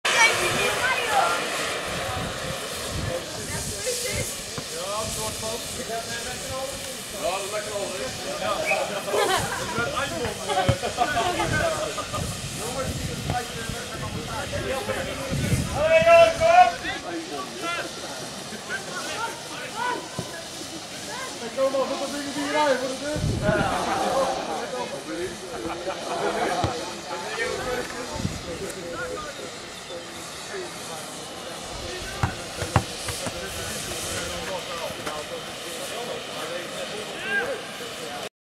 Bevelanders B1 - WIK'57 B1 (26-04-2012)
was leuk
Een fragment uit de wedstrijd Bevelanders B1 tegen WIK'57 B1 op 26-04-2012.
De wedstrijd werd gespeeld op Sportpark Campen te KAMPERLAND en eindigde in een 1 - 3 winst voor WIK'57 B1.